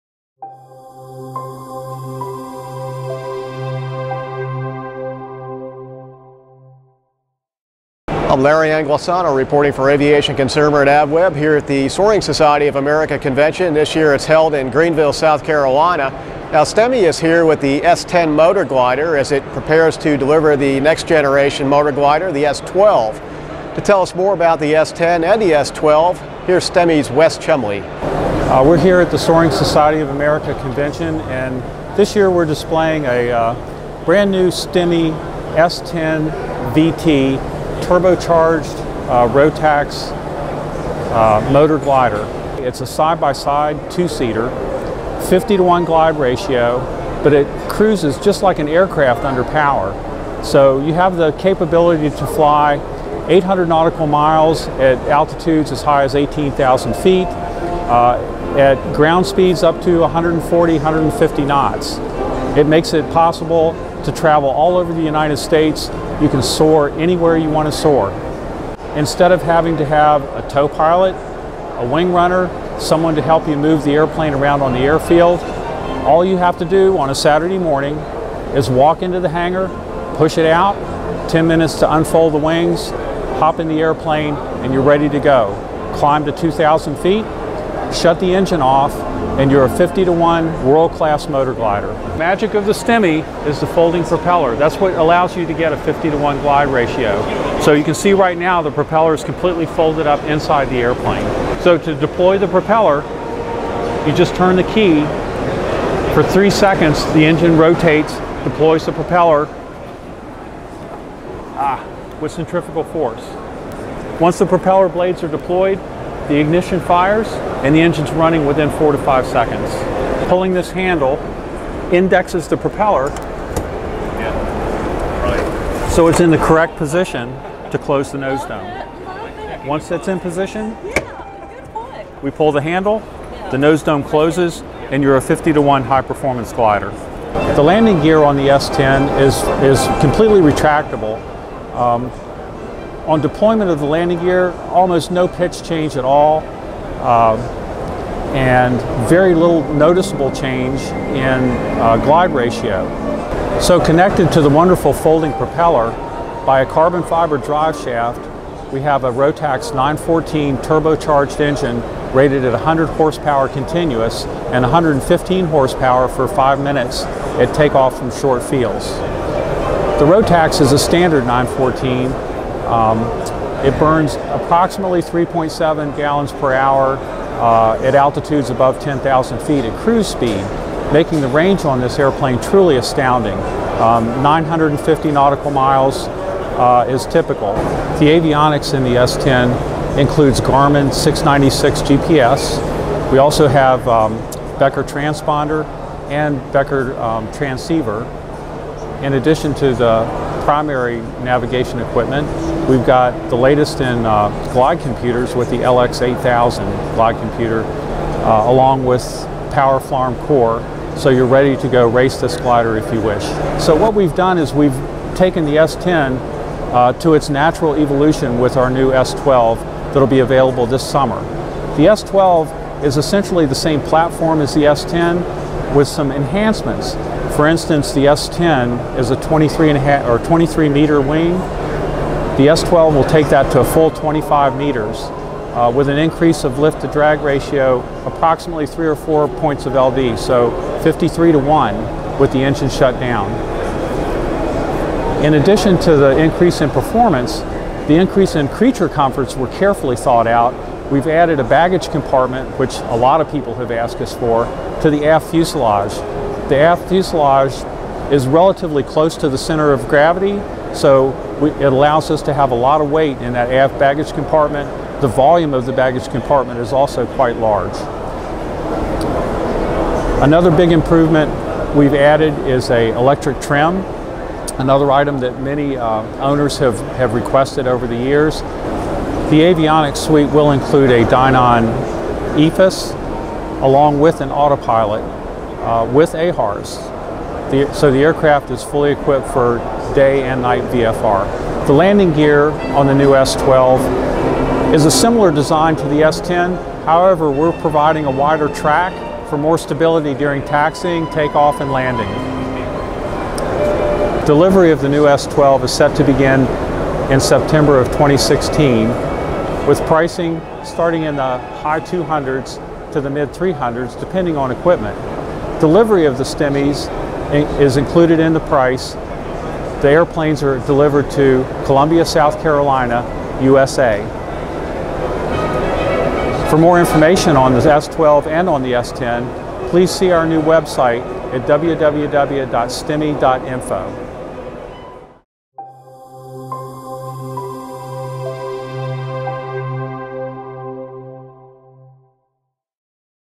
Stemme S10/12 Touring Motorglider
Powered by a Rotax 914 turbocharged engine, the Stemme S10VT and the in-development S12 represent the latest in touring motorglider technology. With a 50:1 glide ratio, 18,000-foot service ceiling, and day and night VFR capability, the aircraft can be used for competition and long distance travel. "Aviation Consumer" magazine editor Larry Anglisano took a closer look at the aircraft at the Soaring Society of America's national convention in Greenville, South Carolina.